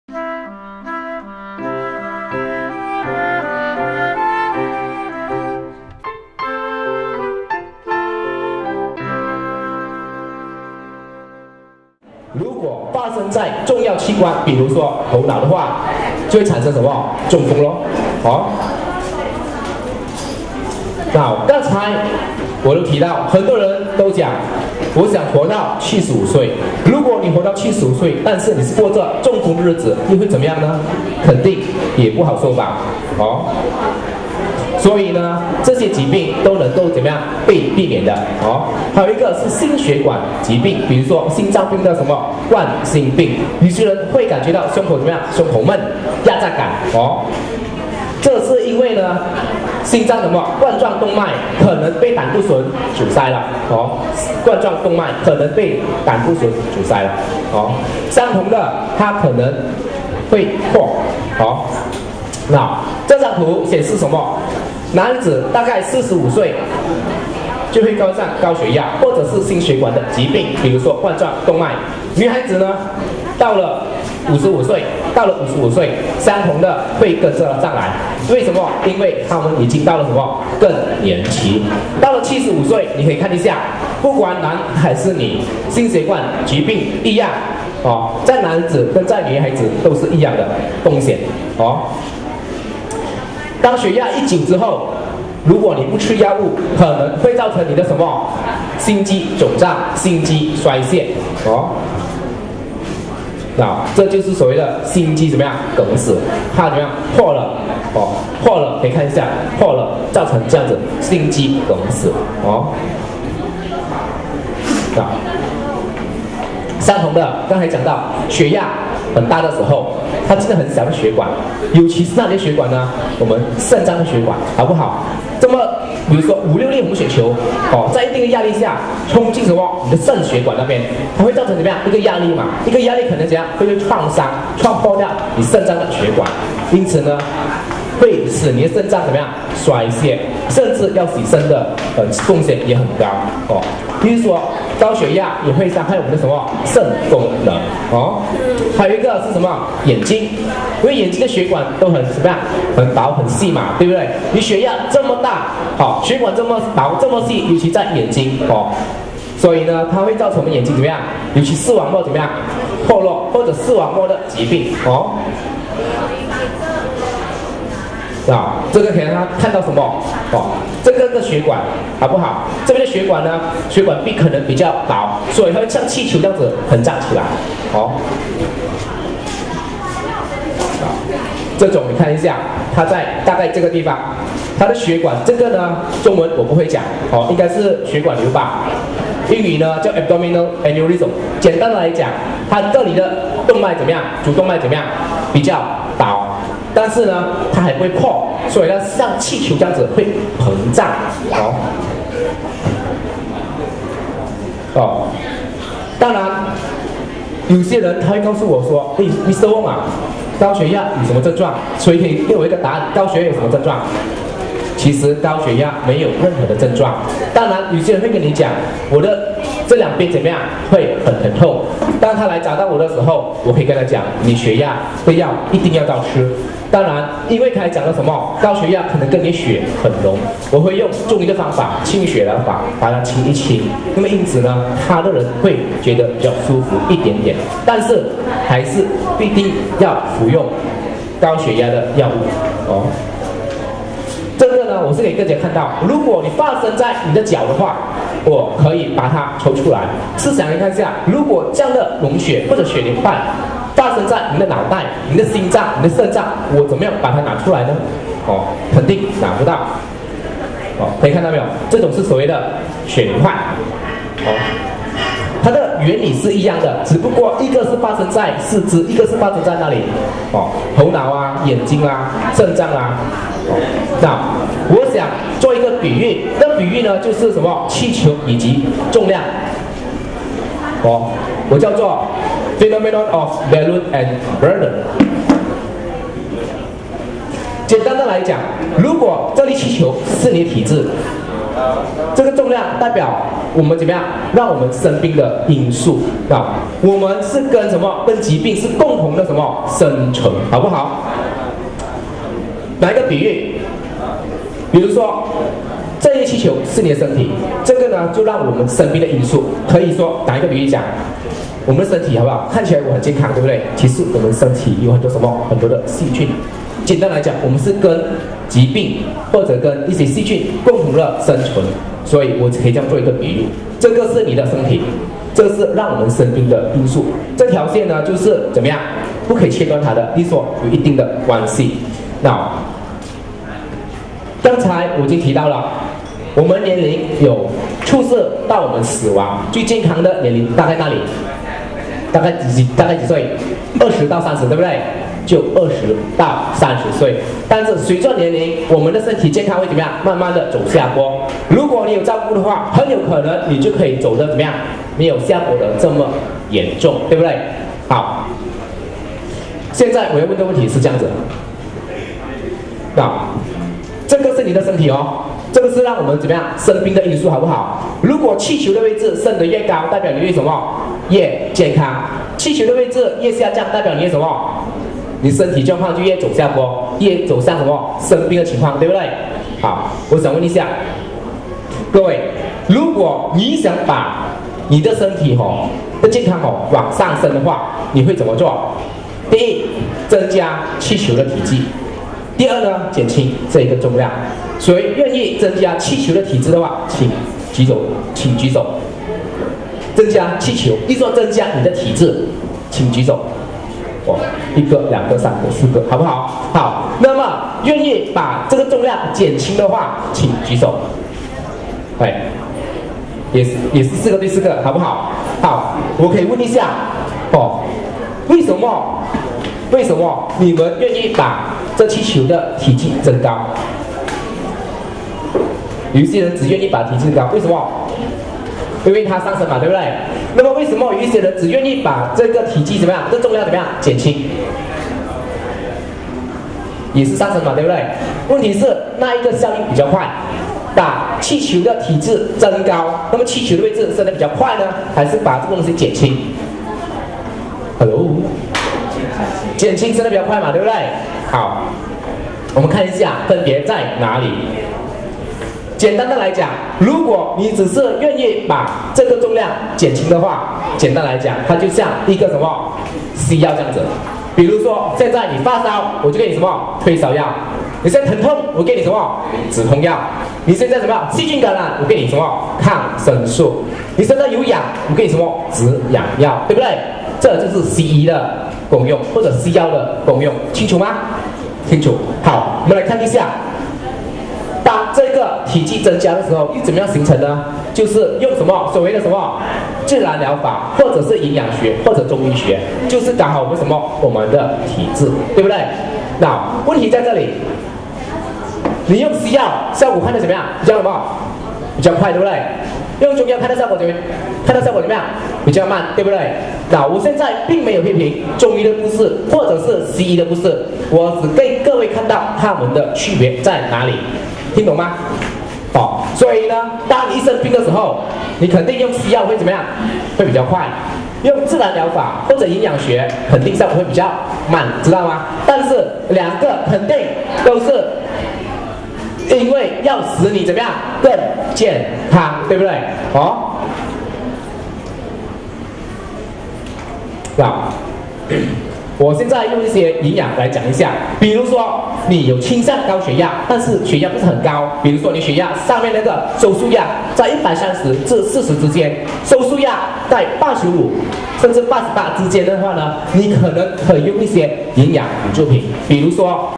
A health talk about Hypertension -Part 2
A health talk about Hypertension by Mr. T.C. Wong